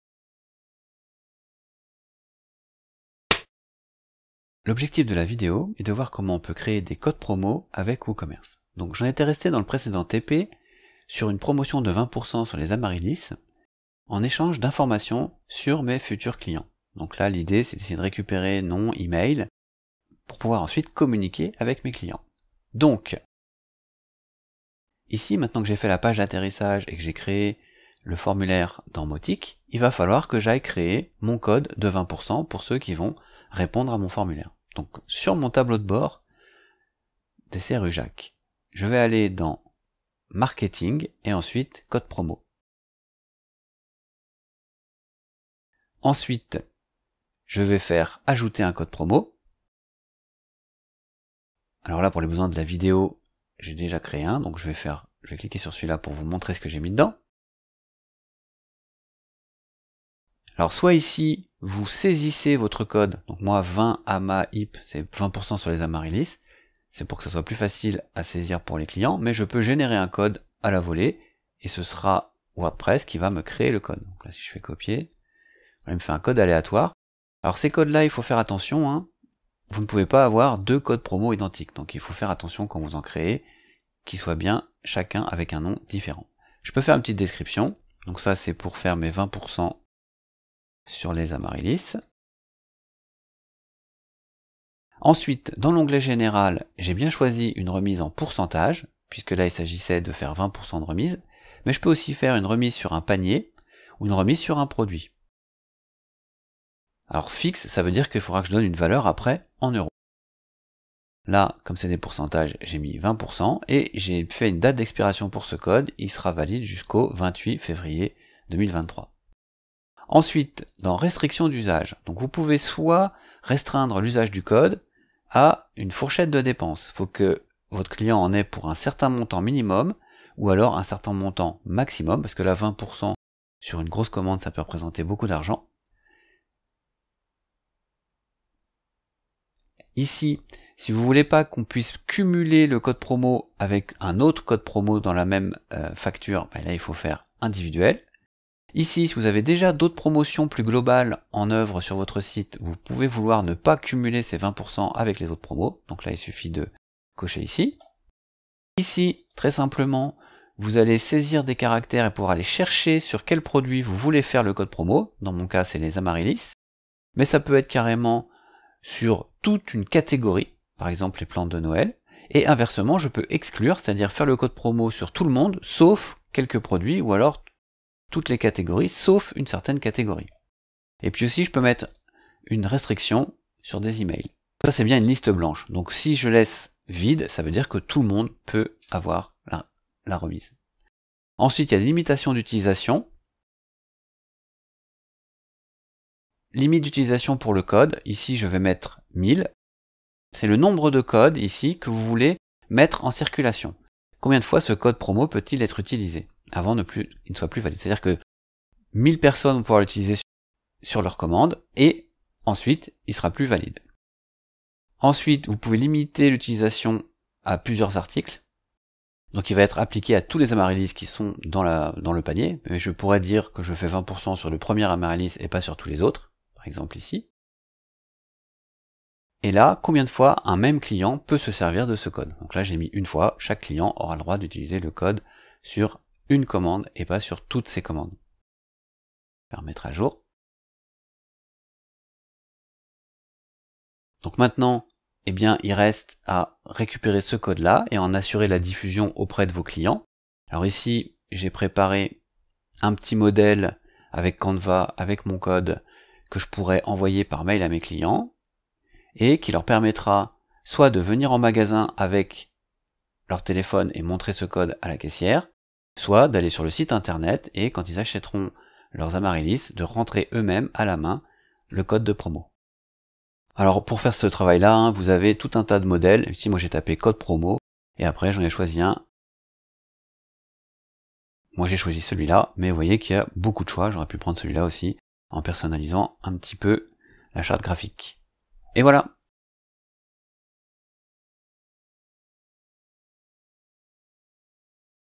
Tuto vidéo : Créer un code promo avec Woocommerce et WordPress
Points à retenir :
- Cliquer sur Marketing - Codes promo
- Ajouter un code promo
- Saisir un code (ou générer)
- Paramétrer la valeur
- Paramétrer les restrictions et les limites